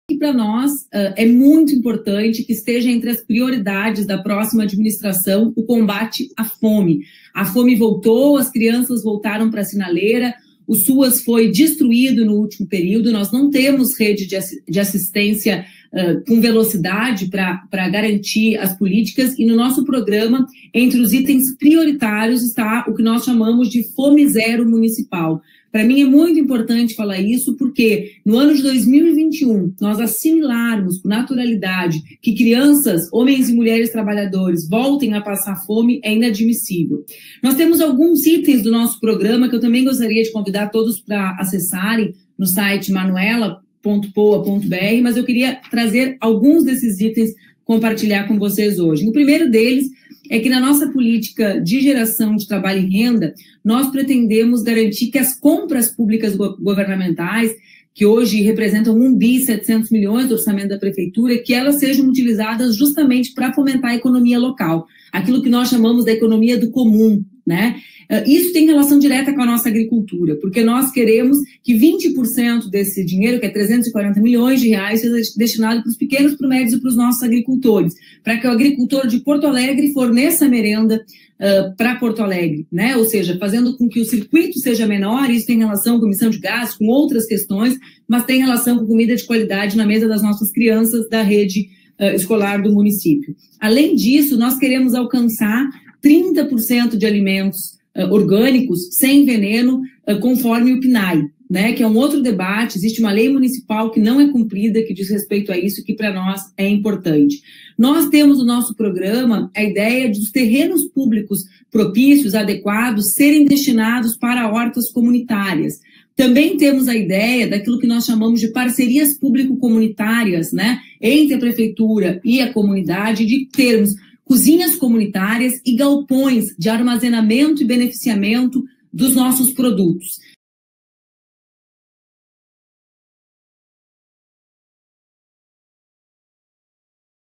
Combater a fome é uma das nossas prioridades | Debate CONSEA RS
Confira minha fala no debate do Conselho de Segurança Alimentar e Nutricional Sustentável do RS (Consea). Entre os itens prioritários do nosso governo, está a fome zero municipal. Não podemos aceitar que homens e mulheres passem fome na nossa cidade. Também queremos garantir comida de qualidade na mesa das crianças da rede municipal. Vamos construir esse caminho.